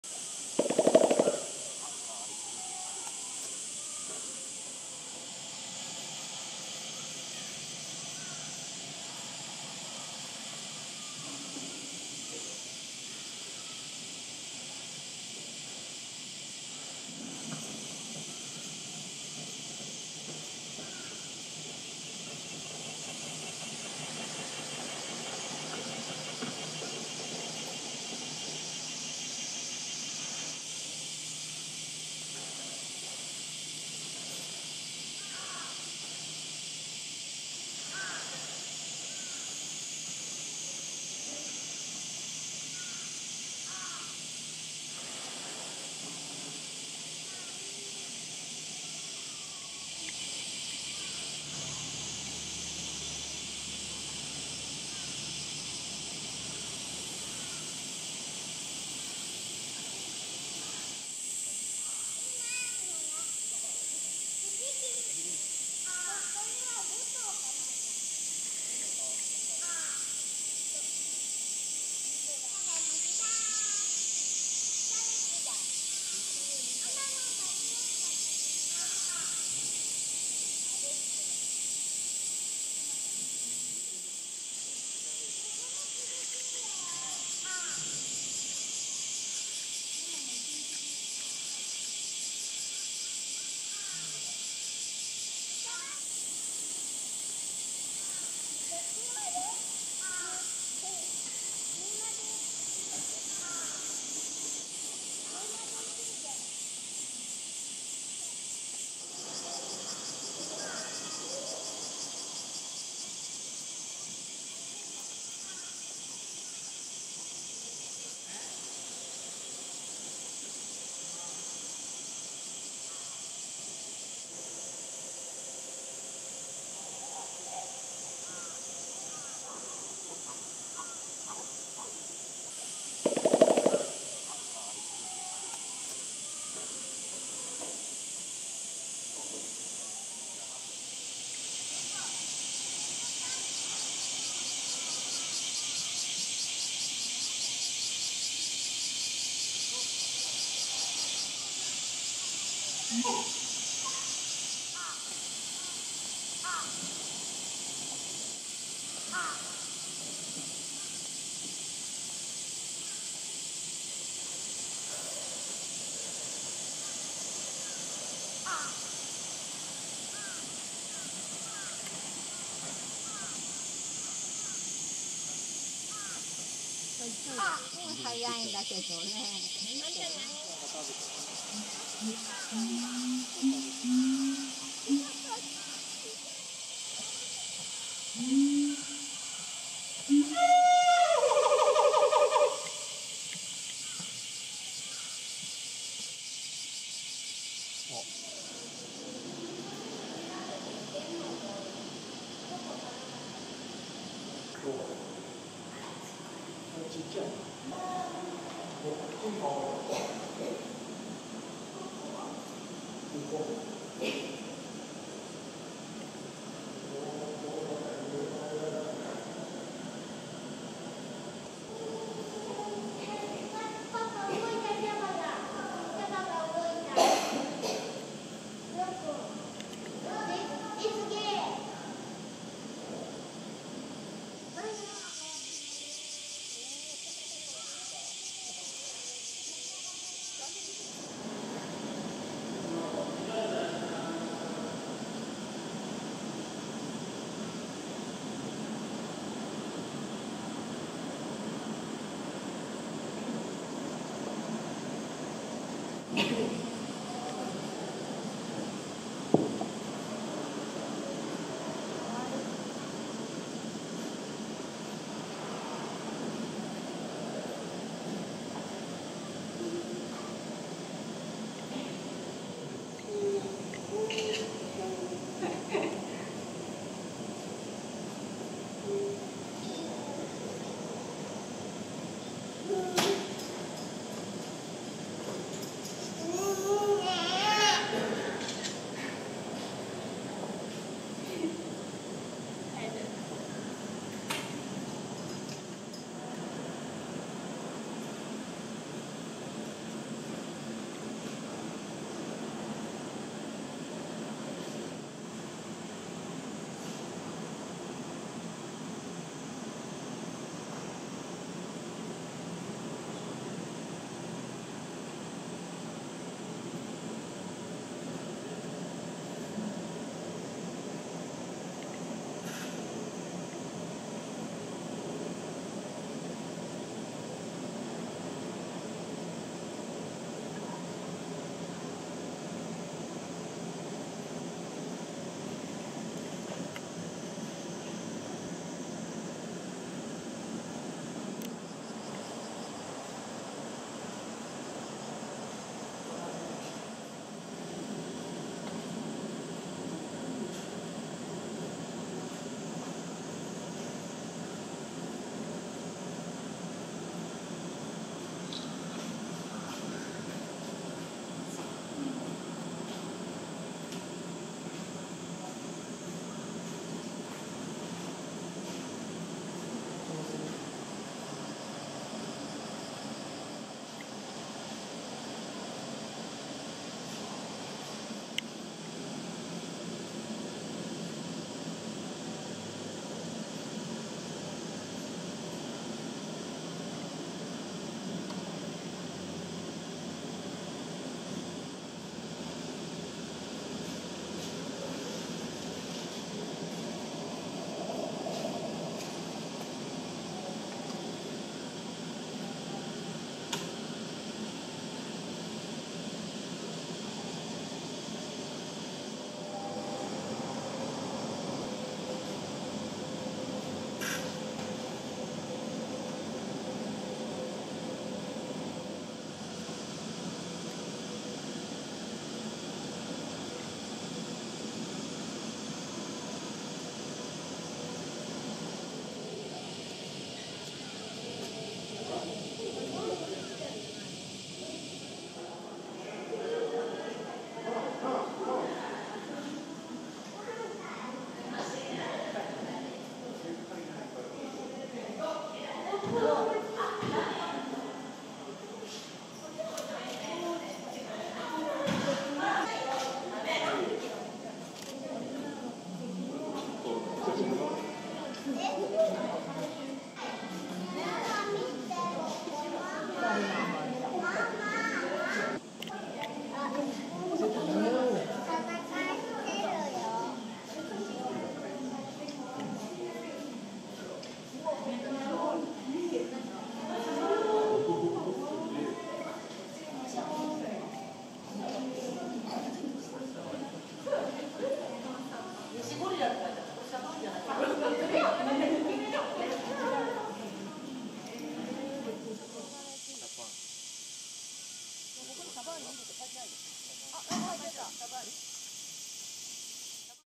Gorillas Who Want To Play Together No Matter How Hot It Gets | The Shabani Group
August 2023.
Higashiyama Zoo on a very hot day.
It was so hot that even humans did not want to move, but the fur-clad gorillas must have been even hotter.
But even on such a hot day, the gorillas were communicating with each other by touching each other.

Time Stamps
00:00 Breakfast time for gorillas
03:22 Gorillas relaxing indoors
04:35 Kiyomasa
05:03 Annie
06:49 Nene wants to go back inside

Shabani: Male Born October 20, 1996
Nene: Female Born in 1972 (estimated)
Ai: Female Born February 27, 2003
Kiyomasa: Male Born November 1, 2012
Annie: Female Born June 2, 2013